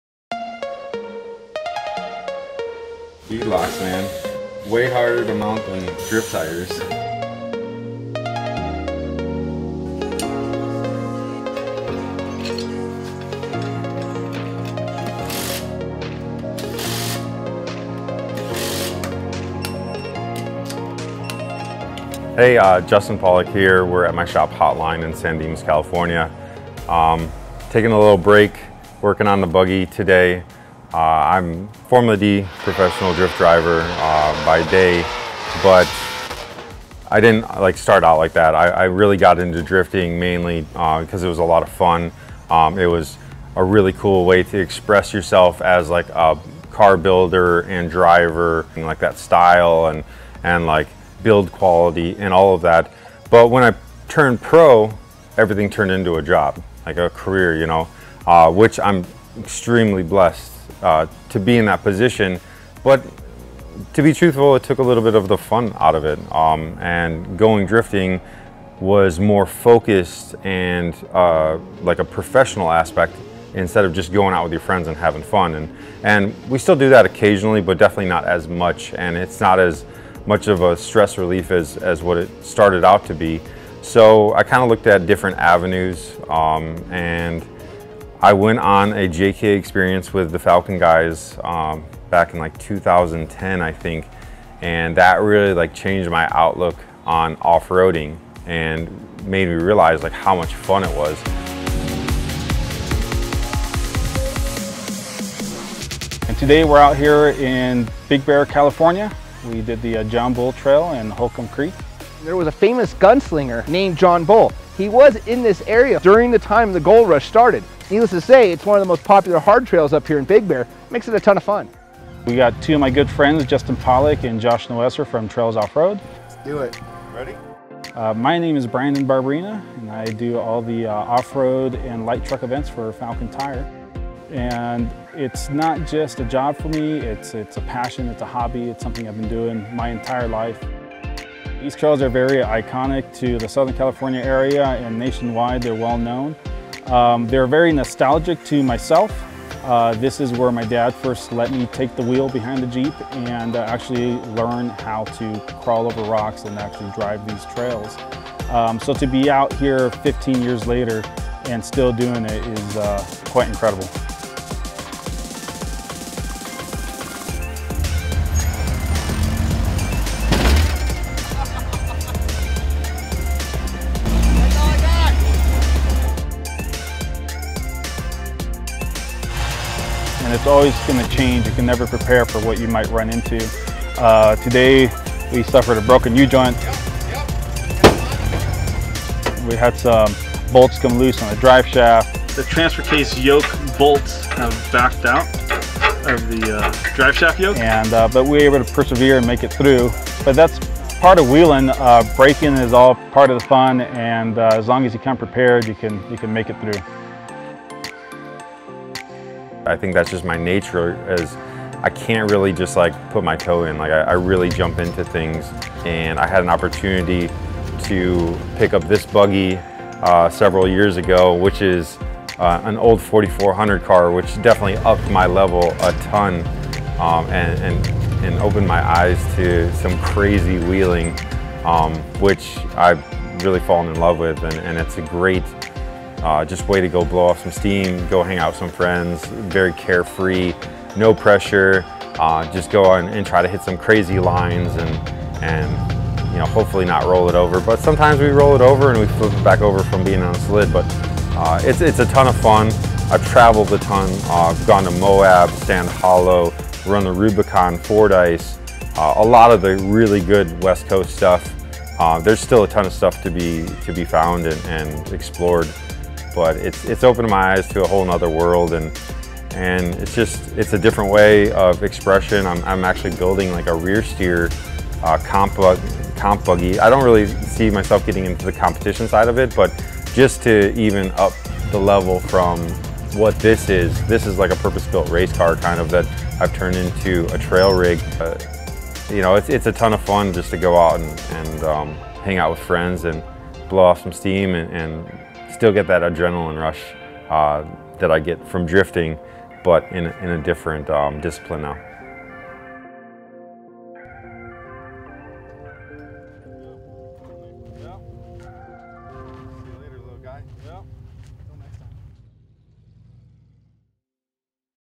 Falken Trail Run with JTP Justin Pawlak
Justin "JTP" Pawlak, a long time member of Falken Tires' professional Formula Drift team, uses rock crawling as a form of stress relief in his downtime. Falken, along with Trailsoffroad.com, hung out with Justin for a day of exploring some of the San Bernardino National Forest's most iconic off-road trails.